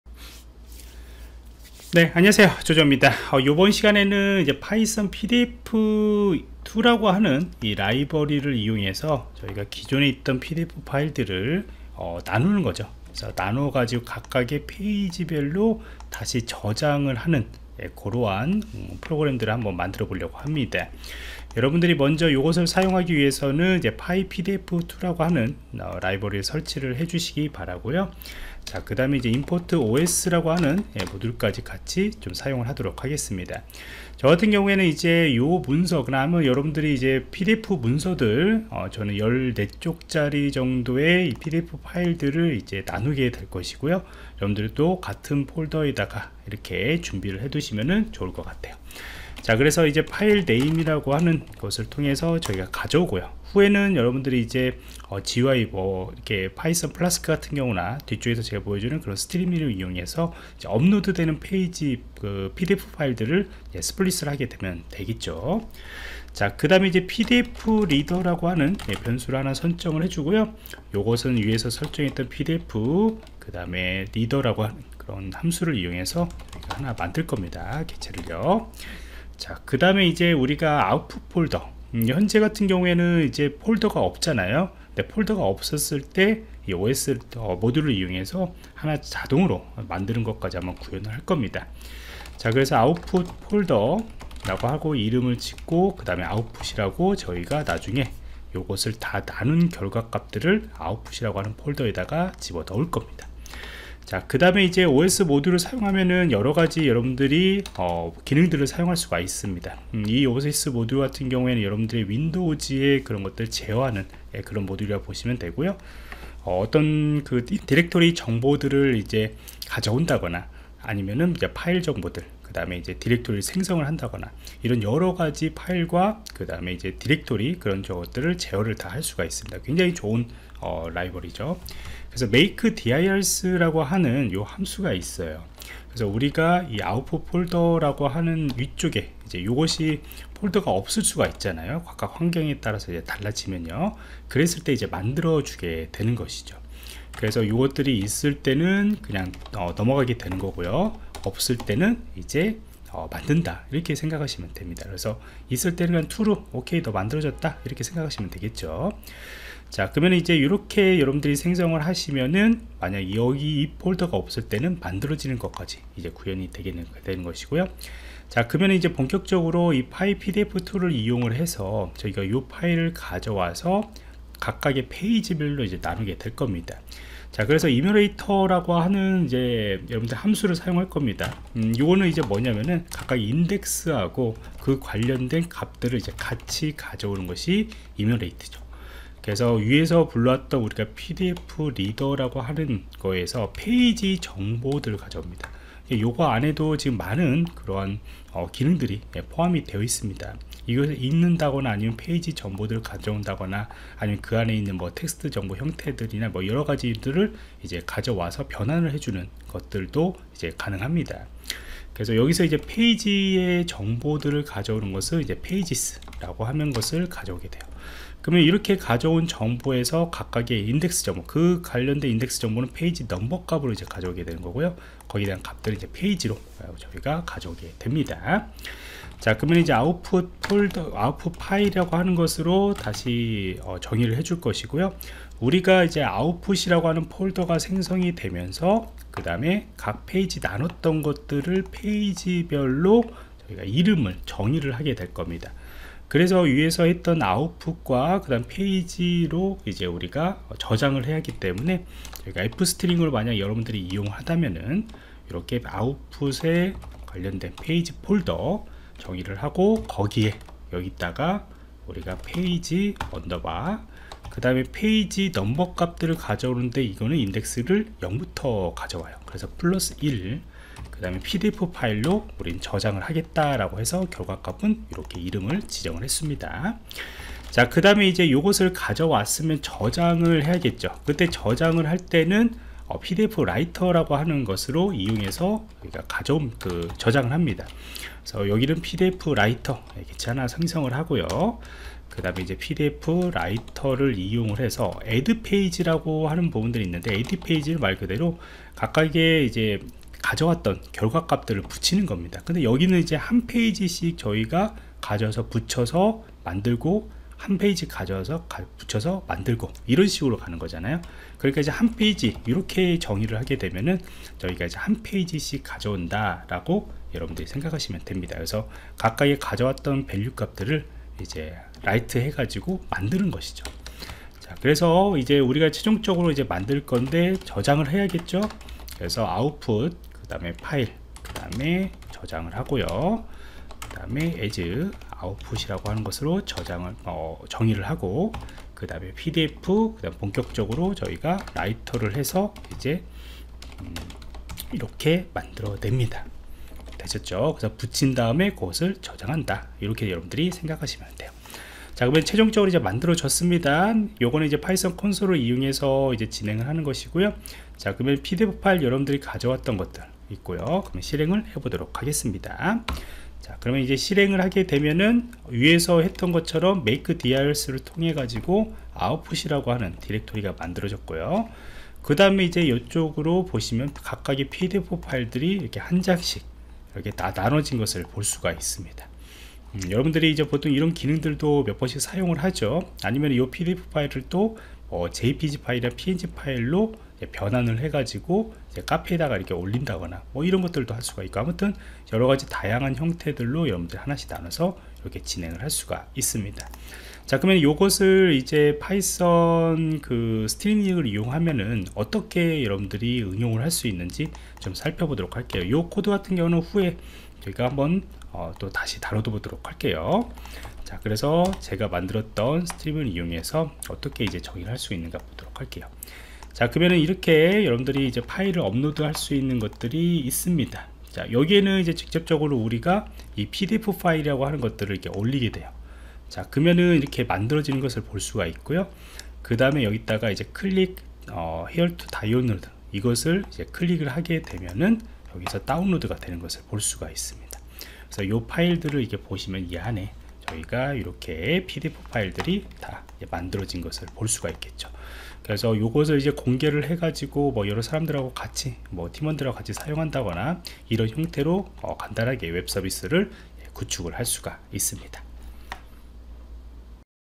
파이썬 PyPDF2 라이브러리를 이용한 PDF를 한장씩 자동 나누기
보안프로젝트에서 ) 제공하는 IT 강의입니다. 강의 주제는 "파이썬 PyPDF2 라이브러리를 이용한 PDF를 한장씩 자동 나누기 python 파이썬자동화 " 입니다.